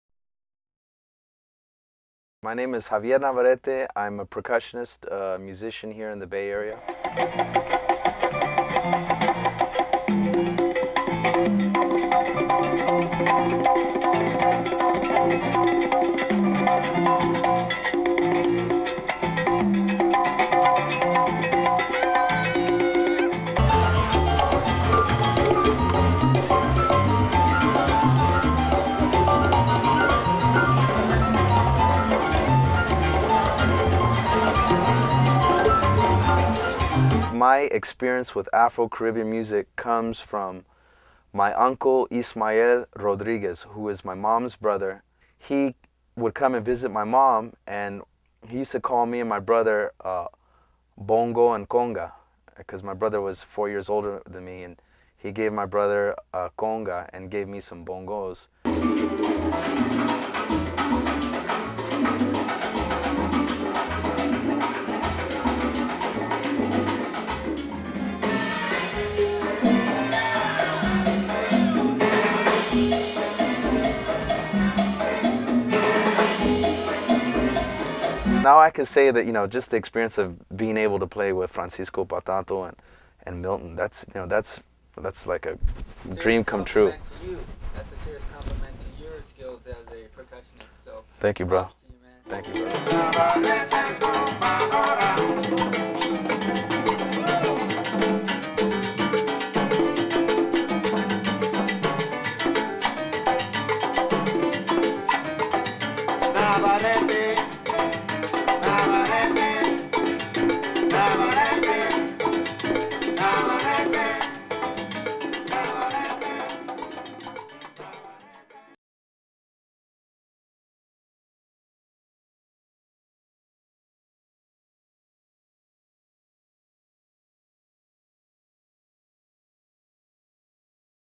Javier Navarrette Intro & Bio (by Rick Pruett) 2003
Javier Navarrette, Percussionist from the Bay Area, CA bio 2003